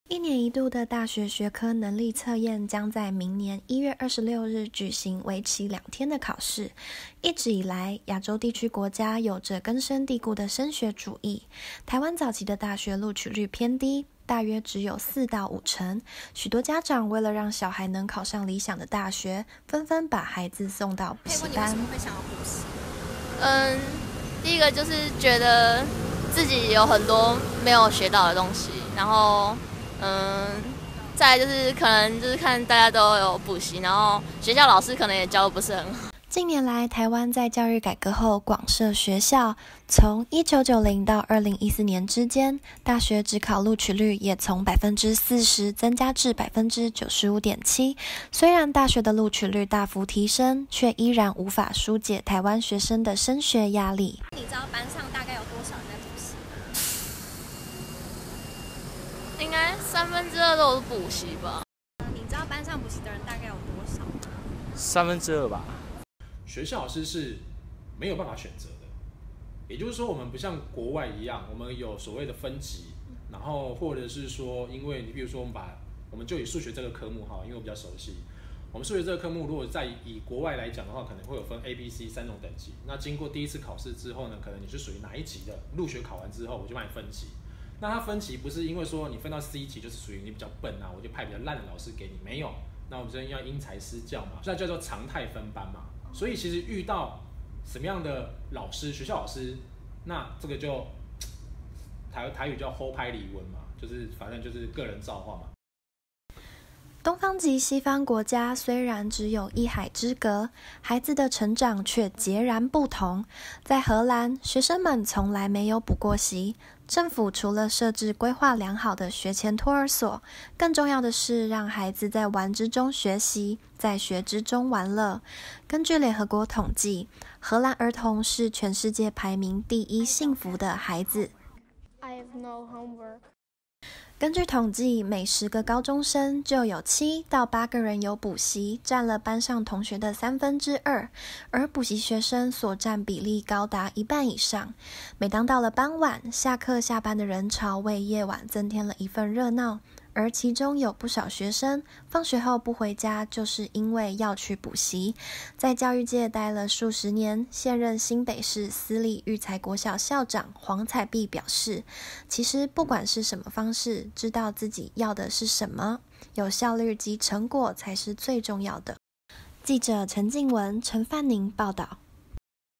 我們共同的夢魘 補習真的有效嗎？
「又到了學測前夕，看著下課後就往補習班跑的高中生不禁看見了某個朋友的影子呀…」，究竟台灣學生為何會依賴補習，我國跟其他國家就有什麼不一樣呢？